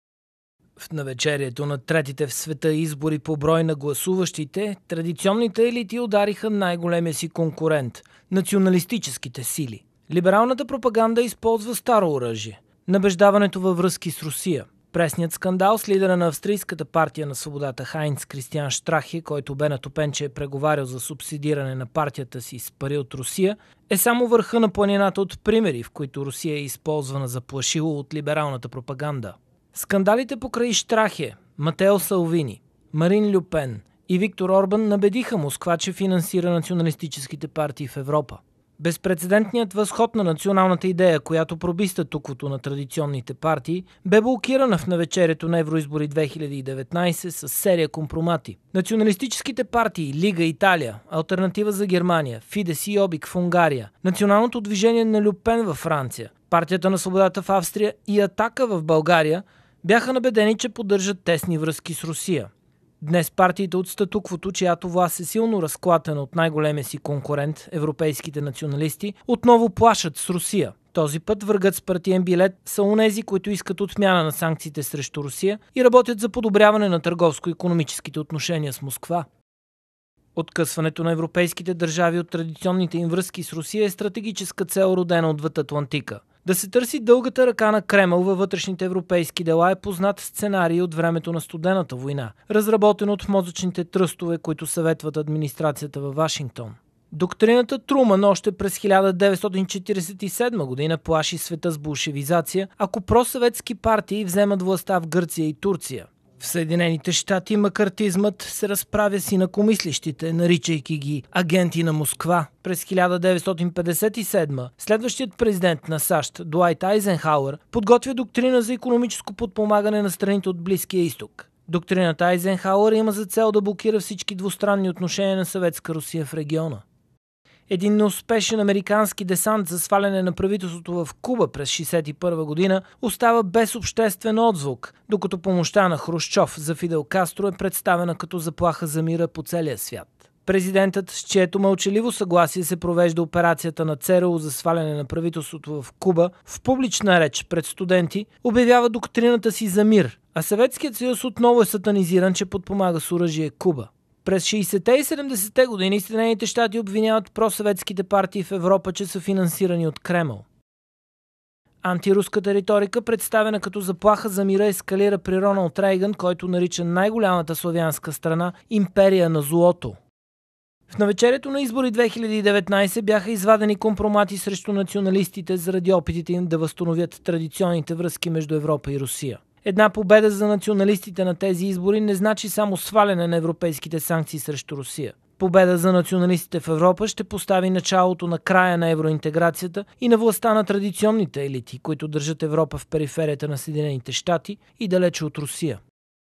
Предизборно. Либералната пропаганда набеждава национализма във връзки с Русия
Анализ на Боян Аспарухов